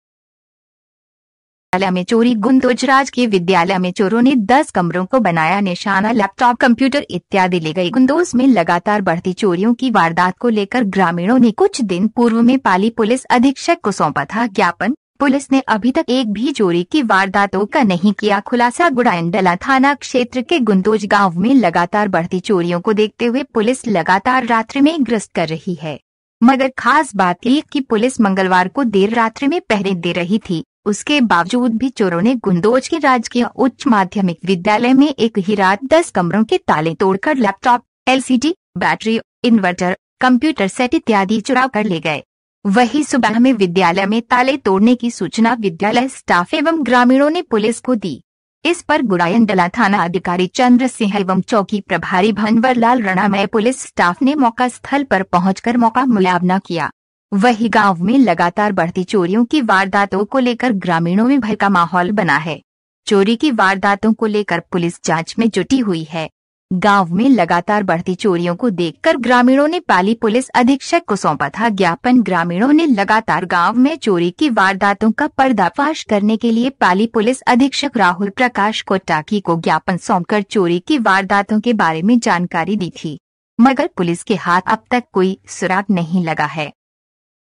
पाली-गुंदोज में पुलिस के पहरे में स्कूल में चोरो ने किए हाथ साफ।
पुलिस के पहरे में गुंदोज विद्यालय में चोरी

गुंदोज राजकीय विद्यालय में चोरों ने 10 कमरों को बनाया निशाना लैपटॉप कंप्यूटर इत्यादि ले गए

गुंदोज में लगातार बढ़ती चोरियों की वारदात को लेकर ग्रामीणों ने कुछ दिन पूर्व में पाली पुलिस अधीक्षक को सौंपा था ज्ञापन , पुलिस ने अभी तक एक भी चोरी की वारदातो का नहीं किया खुलासा

गुड़ा एंडला थाना क्षेत्र के गुंदोज गांव में लगातार बढ़ती चोरियों को देखते हुए पुलिस लगातार रात्रि में ग्रस्त कर रही है। मगर खास बात यह कि पुलिस मंगलवार को देर रात्रि में पहरे दे रही थी, उसके बावजूद भी चोरों ने गुंदोज के राजकीय उच्च माध्यमिक विद्यालय में एक ही रात 10 कमरों के ताले- तोड़कर लैपटॉप, एलसीडी,बैटरी, इनवर्टर ,कंप्यूटर सेट इत्यादि चुराकर ले गए। वही सुबह में विद्यालय में ताले तोड़ने की सूचना विद्यालय स्टाफ एवं ग्रामीणों ने पुलिस को दी। इस पर गुड़ा एंडला थाना अधिकारी चंद्रसिंह एवं चौकी प्रभारी भंवरलाल राणा मय पुलिस स्टाफ ने मौका स्थल पर पहुंचकर मौका मुयावना किया। वही गांव में लगातार बढ़ती चोरियों की वारदातो को लेकर ग्रामीणों में भय का माहौल बना है। चोरी की वारदातों को लेकर पुलिस जांच में जुटी हुई है।

गांव में लगातार बढ़ती चोरियों को देखकर ग्रामीणों ने पाली पुलिस अधीक्षक को सौंपा था ज्ञापन

ग्रामीणों ने लगातार गांव में चोरी की वारदातों का पर्दाफाश करने के लिए पाली पुलिस अधीक्षक राहुल प्रकाश कोटाकी को ज्ञापन सौंपकर चोरी की वारदातों के बारे में जानकारी दी थी। मगर पुलिस के हाथ अब तक कोई सुराग नहीं लगा है।